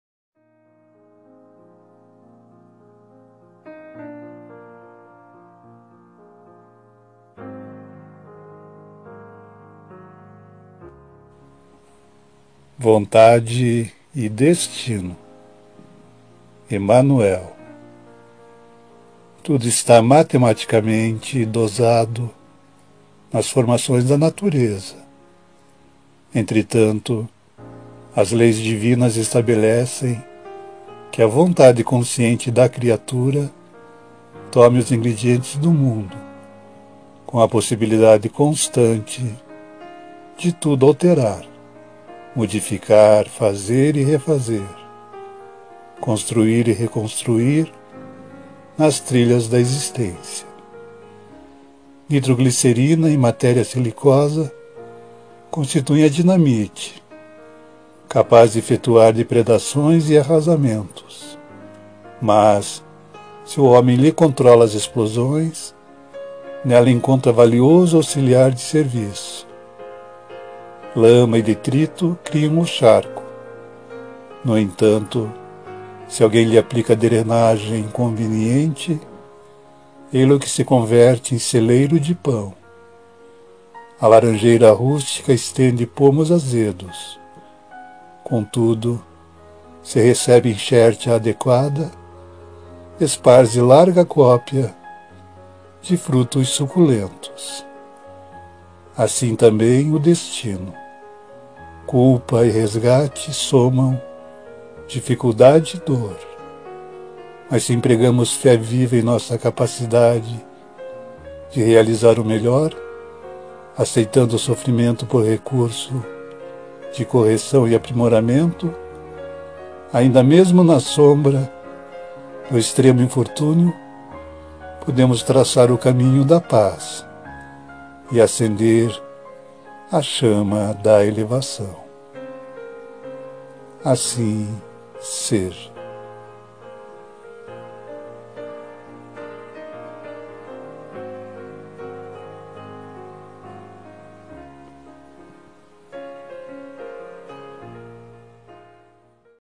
Vontade e Destino! Emmanuel/Chico Xavier! ORAÇÕES E MENSAGENS PARA TODOS OS MOMENTOS!
Por: Emmanuel, Do livro: Trevo de Ideias, Médium: Francisco Cândido Xavier.
Vontade e destino
(Emmanuel/Chico Xavier)

"Tudo está matematicamente dosado nas formações da natureza, entretanto, as leis divinas estabelecem que a vontade consciente da criatura tome os ingredientes do mundo, com a possibilidade constante de tudo alterar, modificar, fazer e refazer, construir e reconstruir nas trilhas da existência.

Nitroglicerina e matéria silicosa constituem a dinamite, capaz de efetuar depredações e arrasamentos, mas, se o homem lhe controla as explosões, nela encontra valioso auxiliar de serviço.

Lama e detrito criam o charco; no entanto, se alguém lhe aplica drenagem conveniente, ei-lo que se converte em celeiro de pão.

A laranjeira rústica estende pomos azedos, contudo, se recebe enxertia adequada, esparze larga cópia de frutos suculentos.

Assim também o destino.
Culpa e resgate somam dificuldade e dor, mas se empregamos fé viva em nossa capacidade de realizar o melhor, aceitando o sofrimento por recurso de correção e aprimoramento, ainda mesmo na sombra do extremo infortúnio, podemos traçar o caminho da paz e acender a chama da elevação."